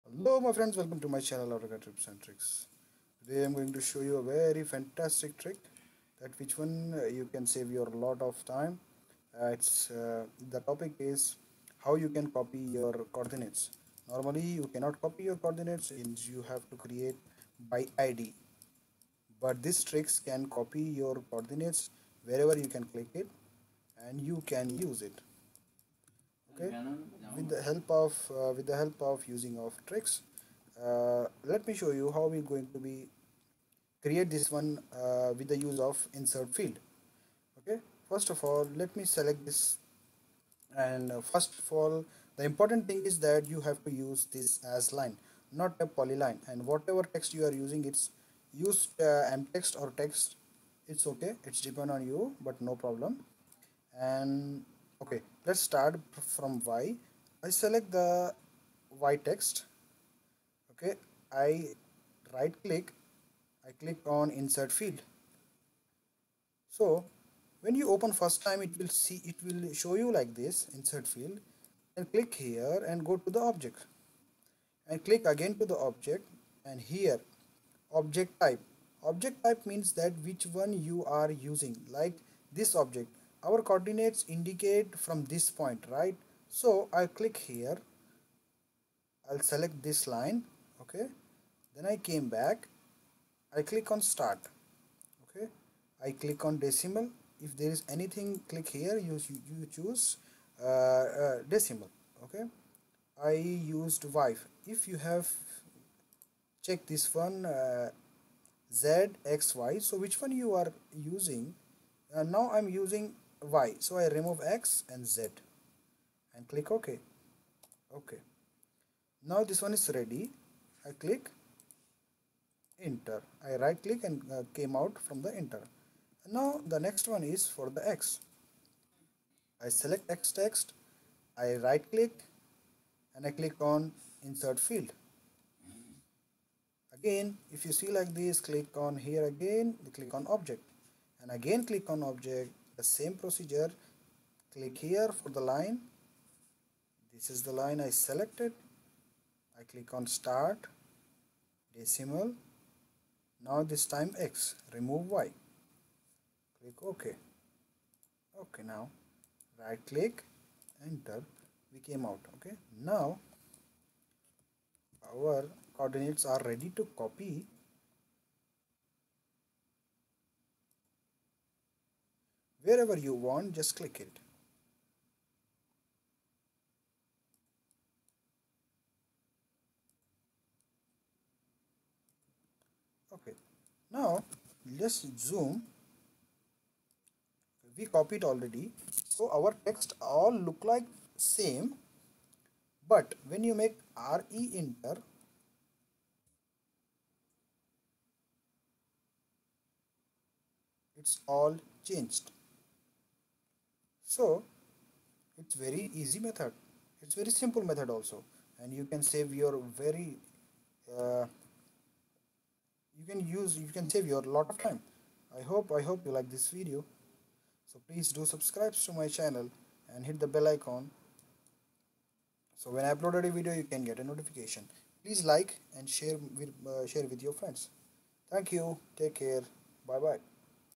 New trick for Automatic Coordinates Creation through insert field 👌😁
Here you can learn how we can make automatic coordinates through insert field we can just copy and click where ever require its very easy method to save your lot of time.

i will bring for you more new tutorials
Thank you so much fro watching for tutorials